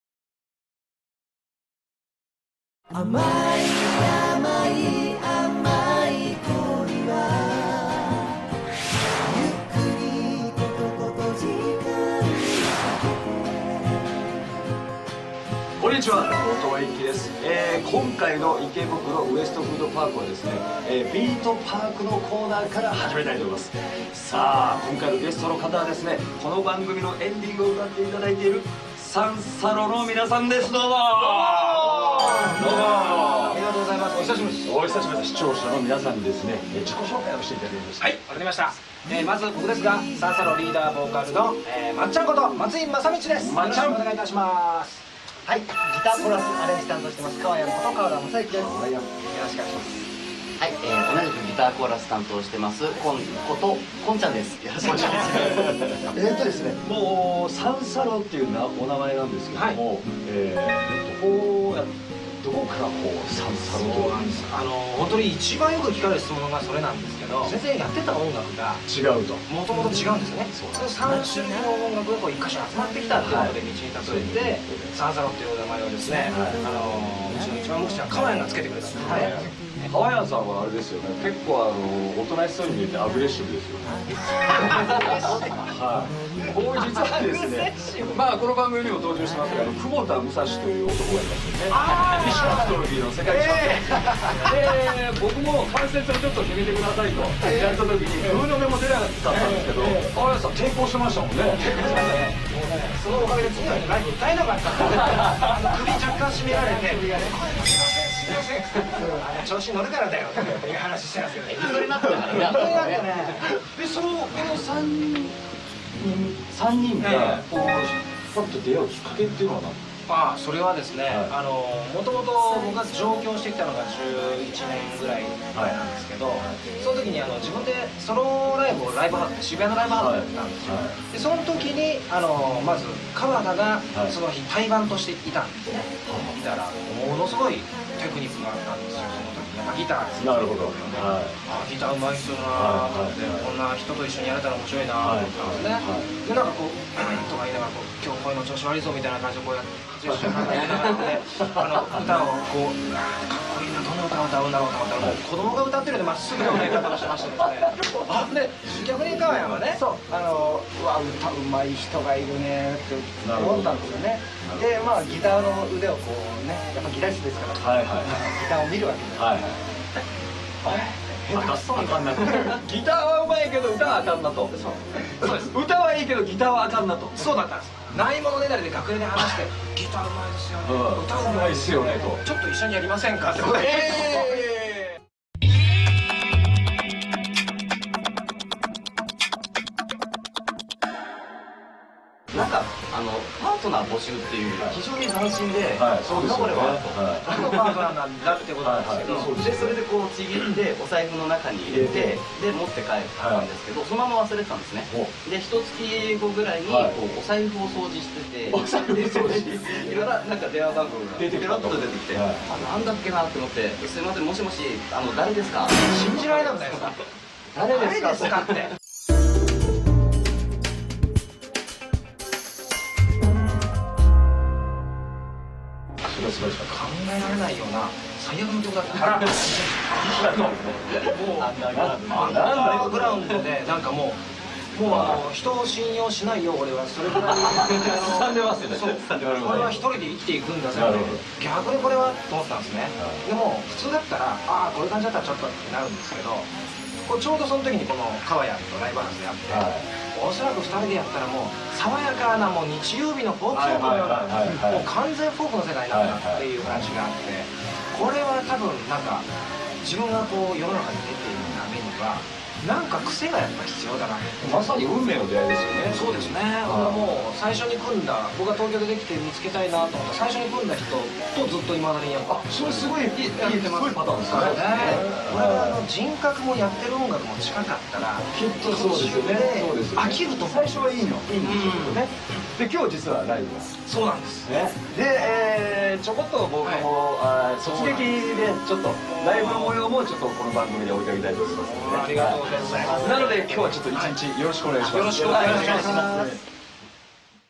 三叉路「結成秘話」
【三叉路】
1999年結成のフォークトリオ。
３声のハーモニーとアコースティックギター3本のスタイルで
2006年「NHK熱唱オンエアバトル」2代目チャンピオンとなり、
同年メジャーデビュー。

メンバー
松井まさみち　Vo/AG
川田雅之　AG/Cho
混xxx　AG/Cho